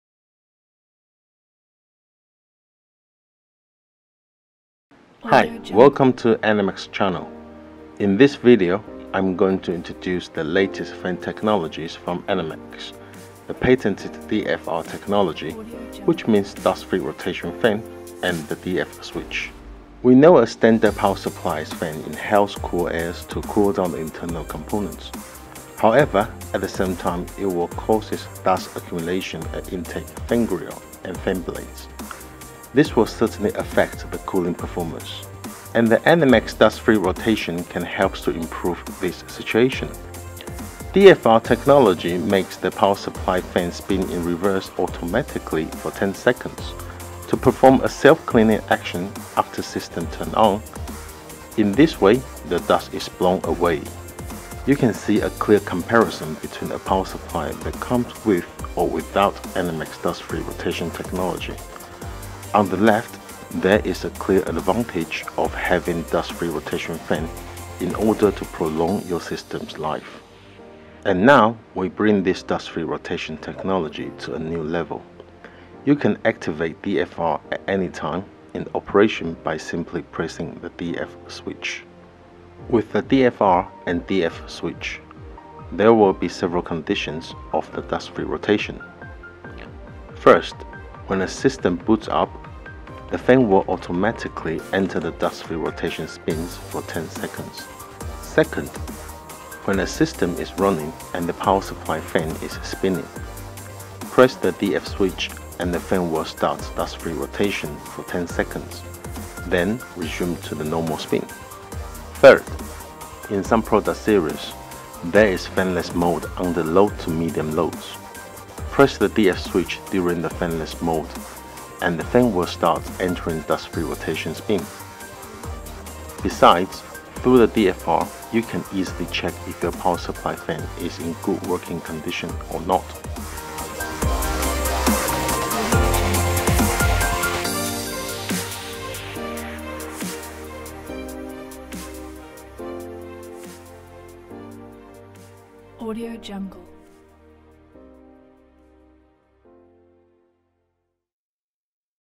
ENERMAX 101 | What is ENERMAX Most Iconic Self-Cleaning Dust Free Rotation Technology?
Dust Free Rotation technology is an innovative self-cleaning solution that allows you to unleash the full potential of your system and ensure a stable power delivery for years. The design will enable the fan inside the power supply to spin smartly, instantly blowing away unwanted dust to prevent any future disruption.

Power Supplies with DFR: MaxTytan / Platimax D.F. / Revolution D.F.
(Revolution D.F.)